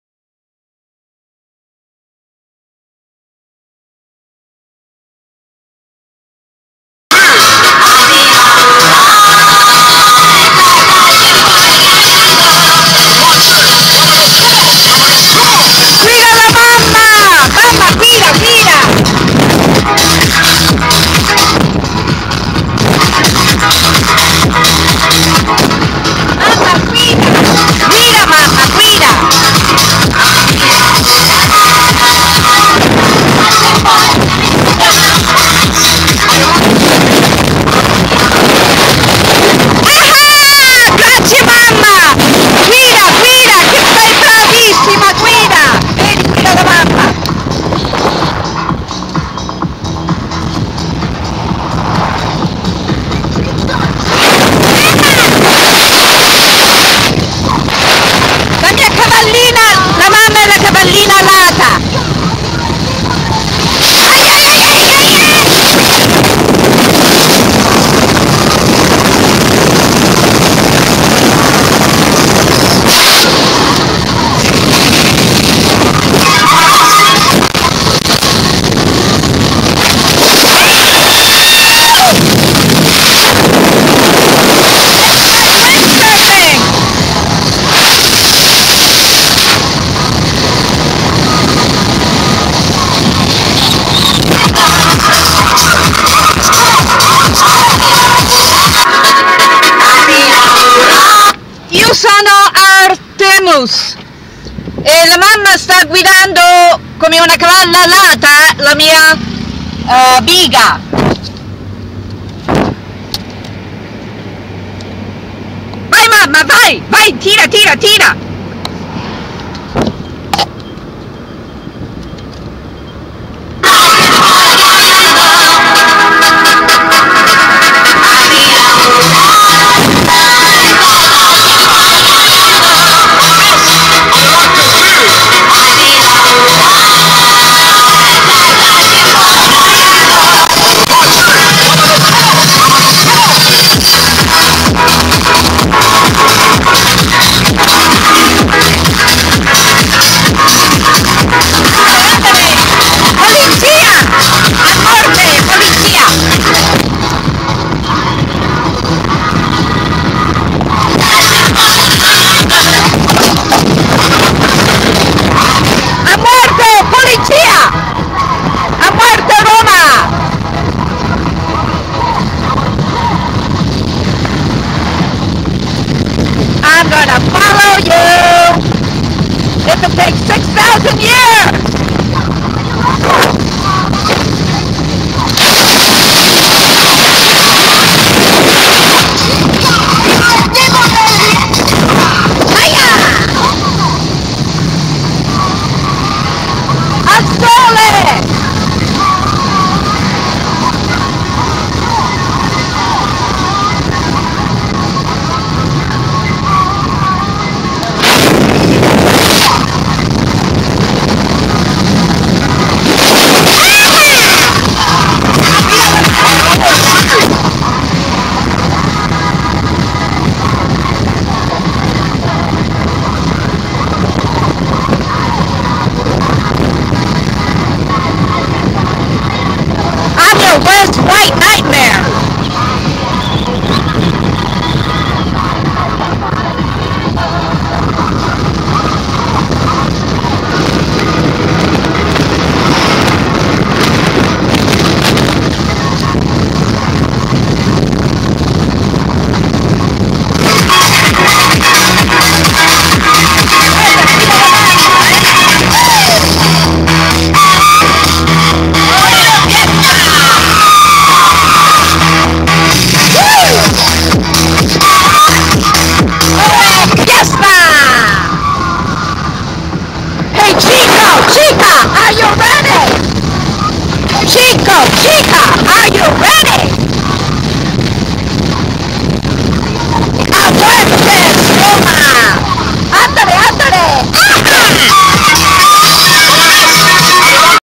Artemus e biga con cavalli alati
Revelazioni di Giovanni la biga e i cavalli alati sono di Artemus la Dea Villanovana e Etrusca cui combatte i Greci (fascisti) e Romani (femminicidiali e cannibali) per salvare l'Etruria e il mondo - guidando una biga richiede piu' talento che il Palio di Siena - uno dei tempii d'Artemus e l'Ara della Regina di Tarquinia, il tempio dove ora c'e' l'amfiteatro romano a Fiesole, e di certo la cava semi marina a Spacco della Regina in Ansedonia